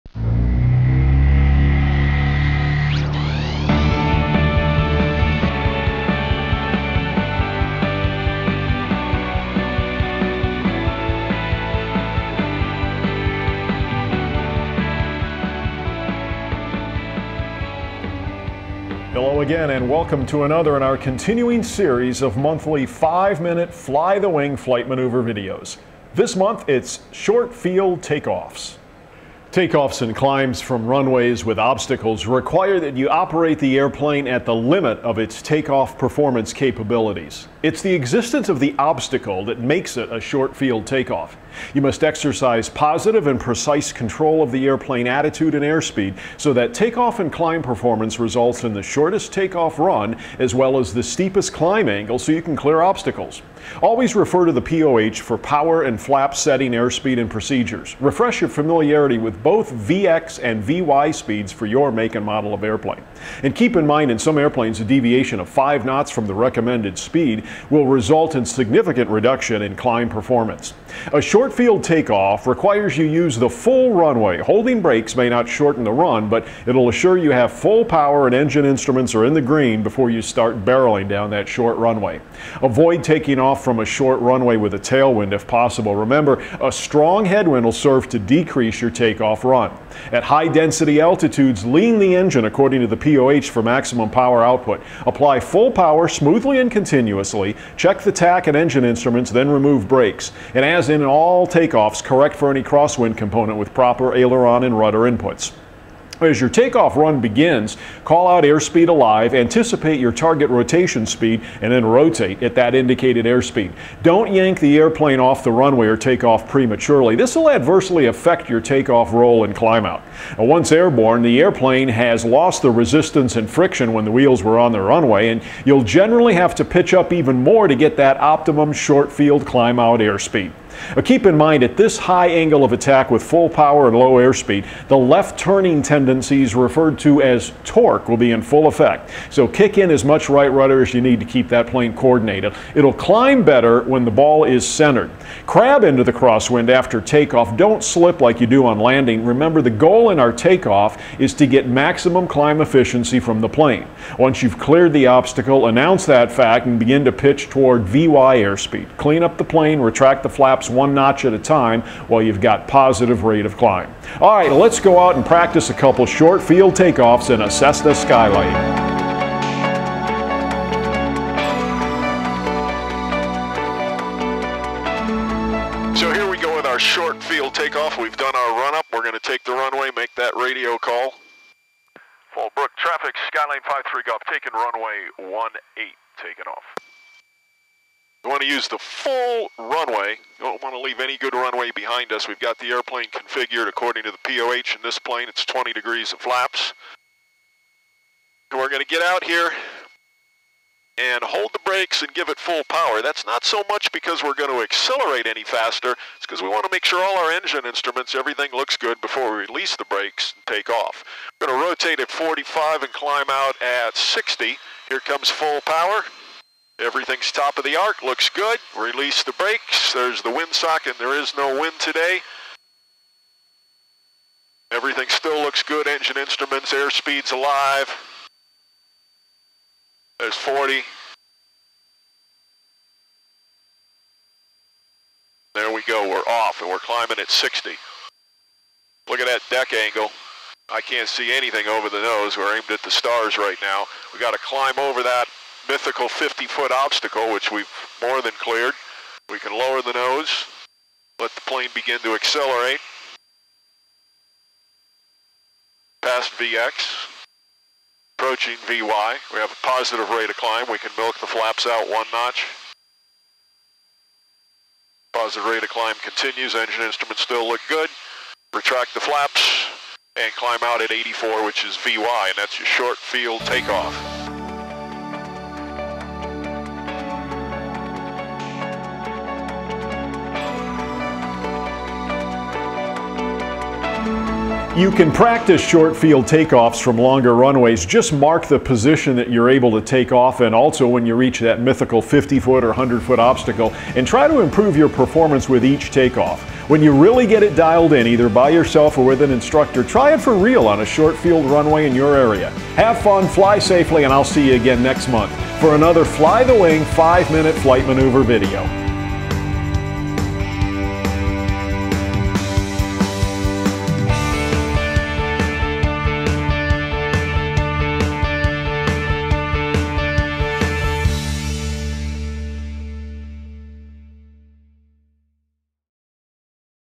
Short Field Takeoff
San Diego Flight Instructor Garry Wing, of Fly The Wing Flight Training, demonstrates the components of a short field takeoff in a Cessna Skylane. View more videos, blog articles and more at the